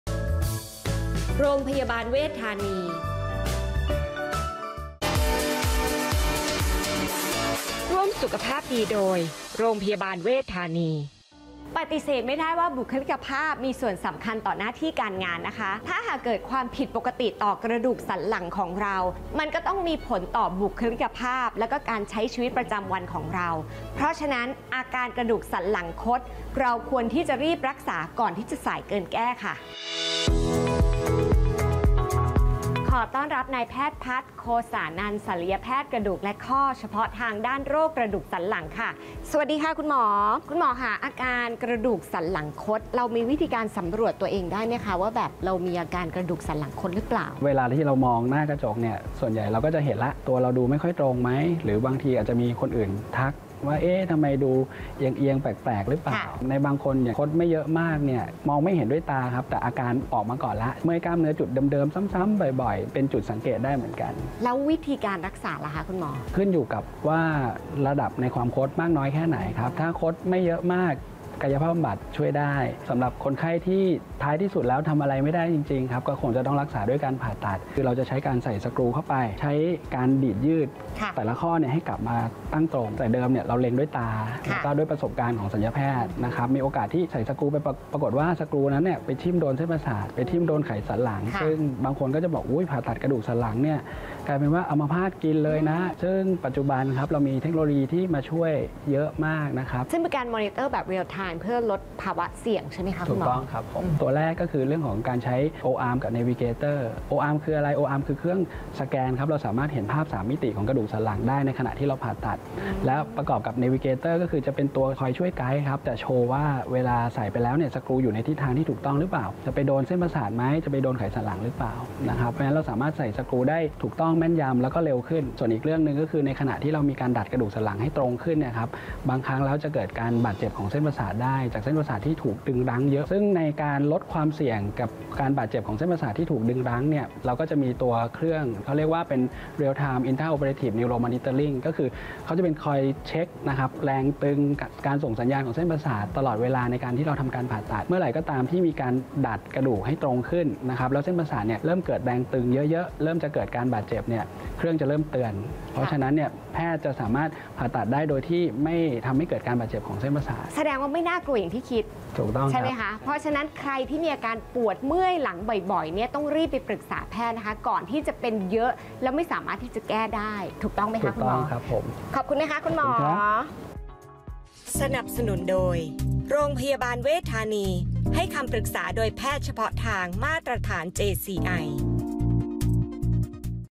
ผ่าตัดกระดูกสันหลังคด ไม่น่ากลัวอย่างที่คิด | Dr.Pattara جرّاح العمود الفقري l โรงพยาบาลเวชธานี
🔻 ติดตามความรู้ด้านสุขภาพกับเราได้ที่